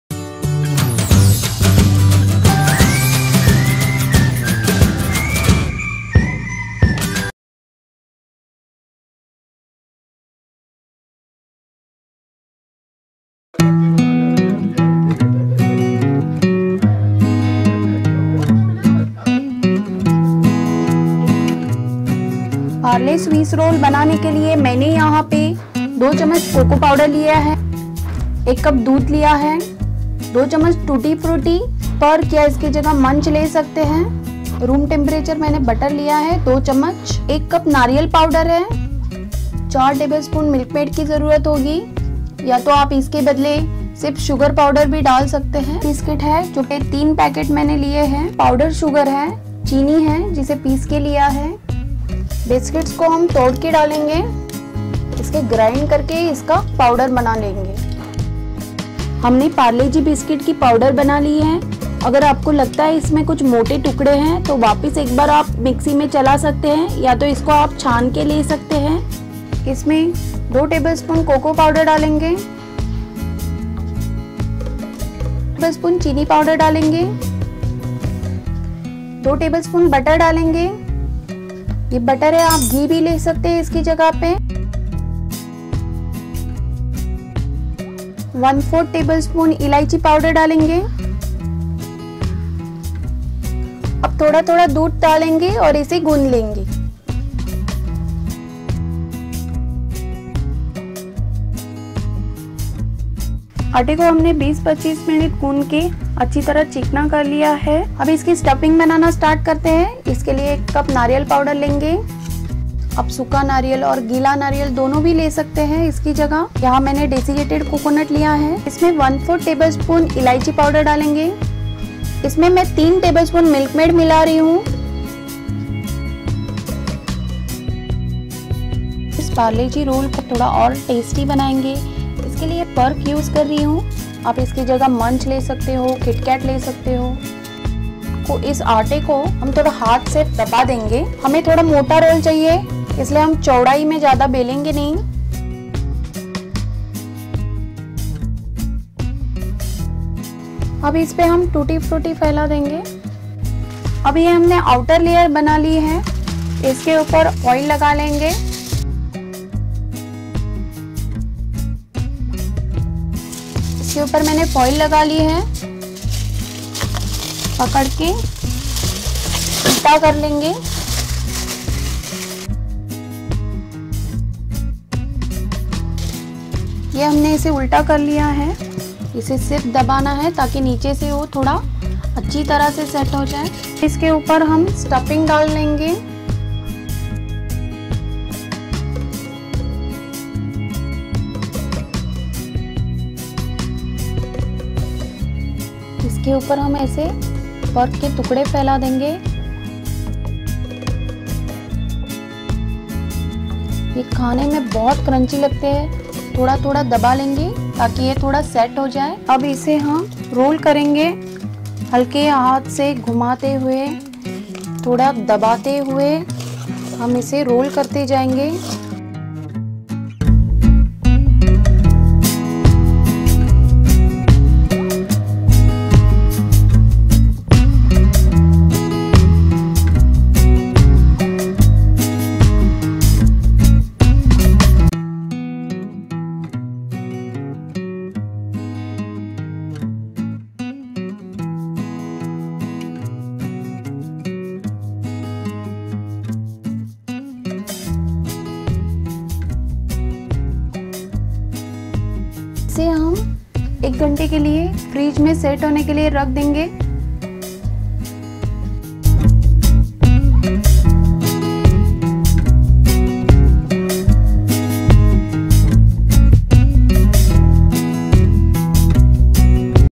पार्ले स्वीस रोल Chocolate Swiss roll /no bake Parle biscuit and Perk swiss roll/mithai recipe
yummy chocolate Swiss roll made with Parle biscuit with included perk crunchy flavour i